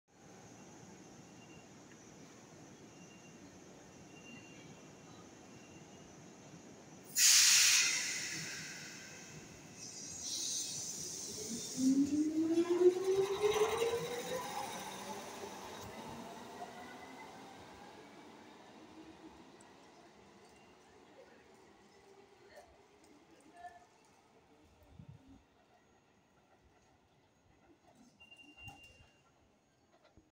【ラスト1編成となった3400形】京成3400形3448編成快速西馬込行き勝田台発車
撮影日:2023.5.4

土休日51K運用
13時27分発車
3438編成が先月14日の運用を以て営業運転終了した模様。残り1編成になったという実感が湧きません…